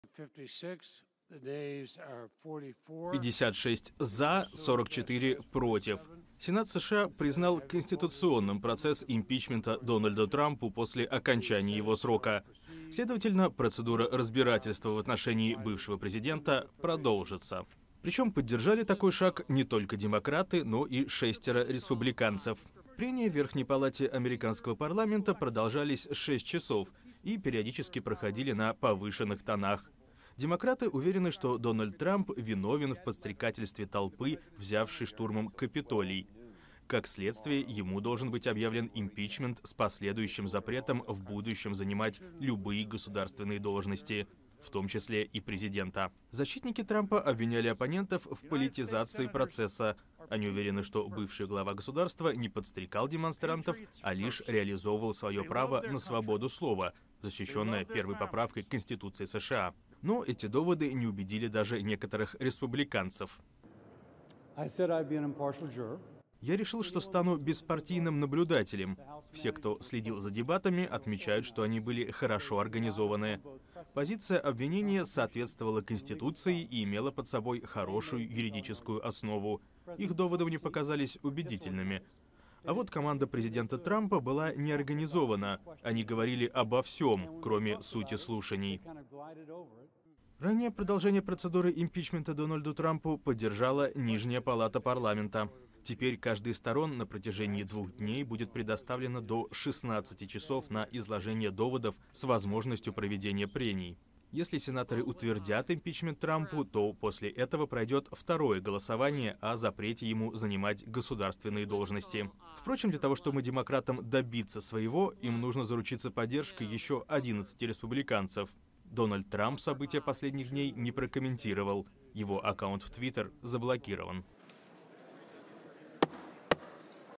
В США начался второй процесс по делу об импичменте Трампа
Сенат США признал конституционным процесс импичмента Дональду Трампу после окончания его срока. Аккаунт Трампа в Twitter останется заблокирован навсегда.